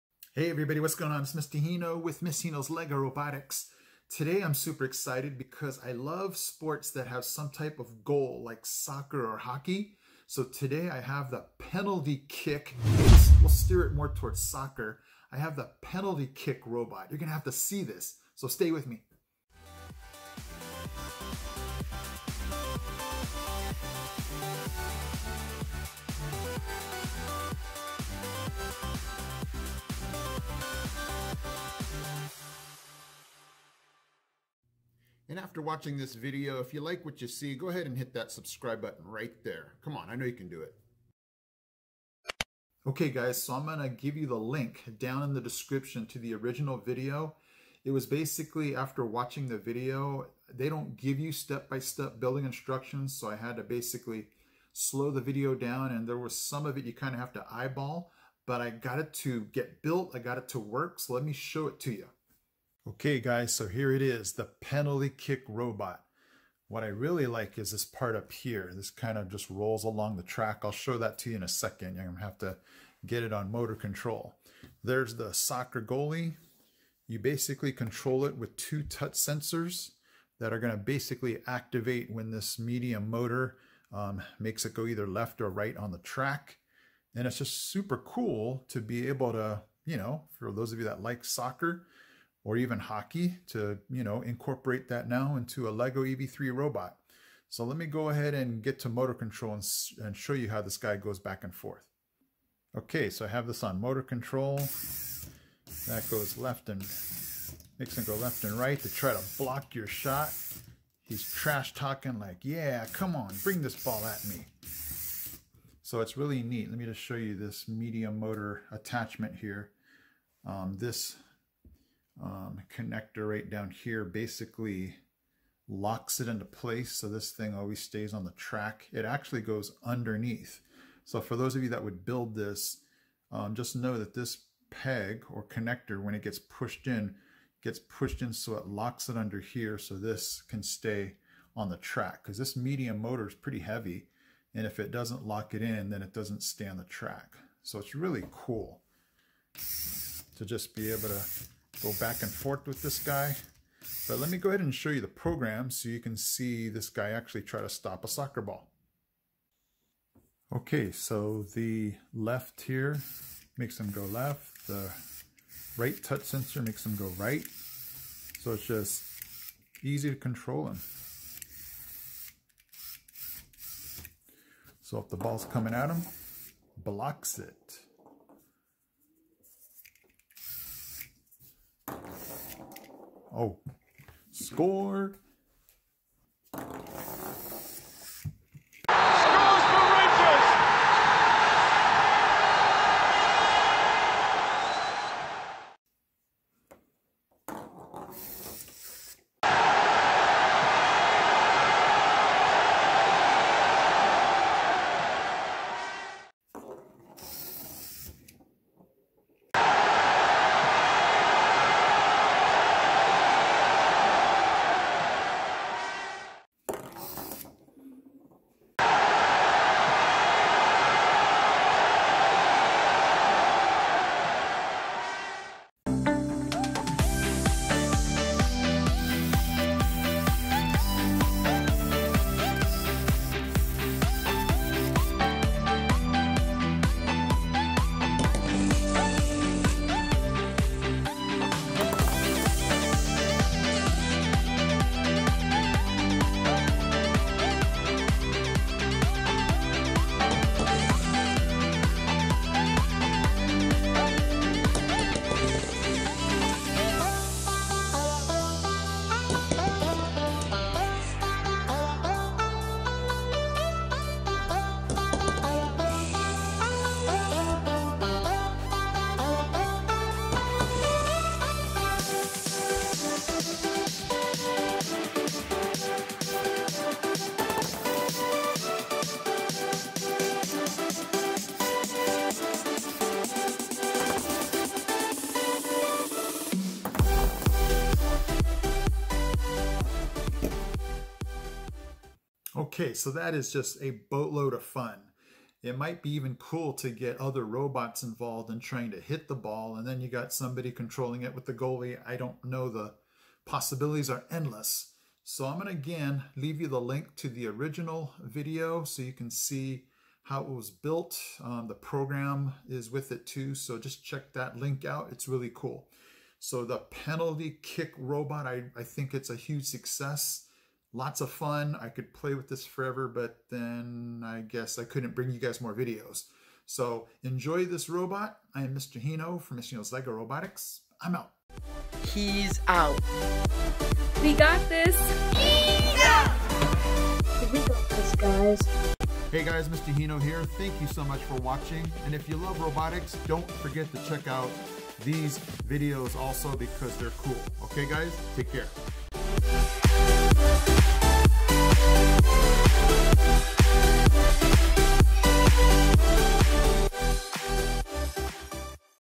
"LEGO EV3 Penalty Kick Robot!!!"
Hey guys!!! This LEGO EV3 "Penalty Kick" Robot is awesome! You can control the goalie with two touch sensors. The goalie moves back and forth by a medium motor clipped to the top bar of the goal. Here is the link for the building instruction and program video. Hope you enjoy!!!